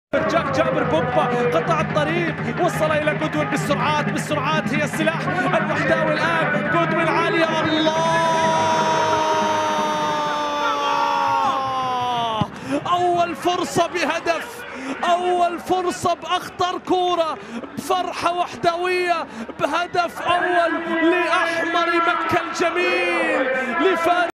هدف الوحدة التعادل في مرمى التعاون 1-1 يوسف نياكاتي - الدوري السعودي 2019-2020
دوري محمد بن سلمان للمحترفين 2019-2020

الجولة 12